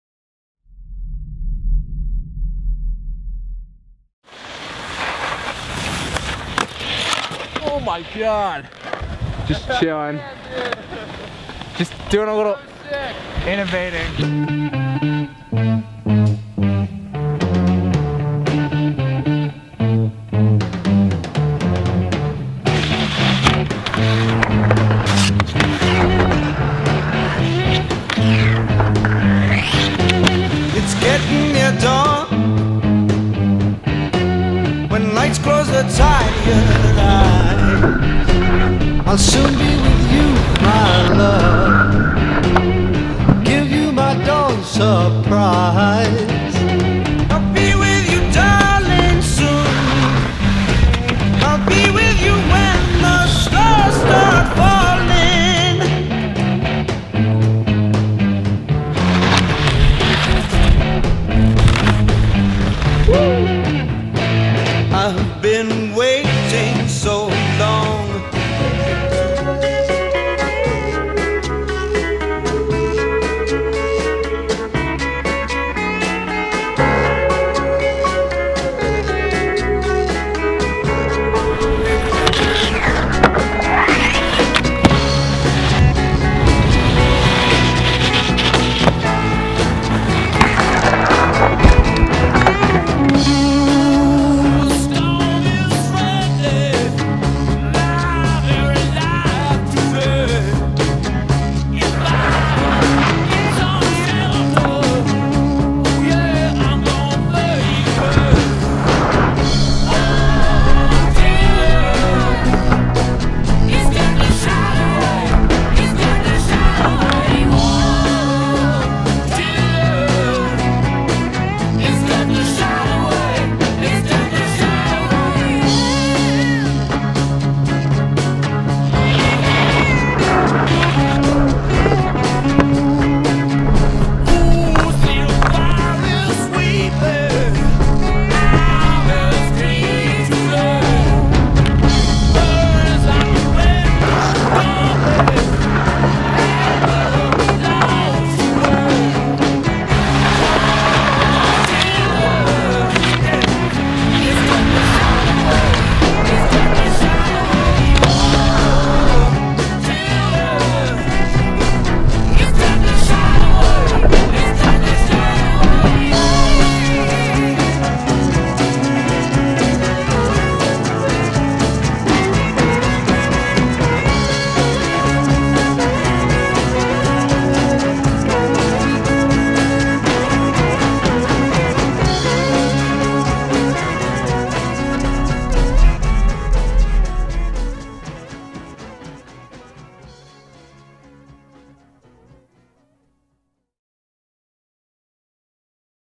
NEW INTEL. - "MCVEIGH"
Spent a week skiing filming with kieran at Sugarbush in early January. Heres what we made of it! Kieran is riding on the Nordica Soul Rider!

Filmed and Edited by Evan Williams
Skiing by Kieran McVeigh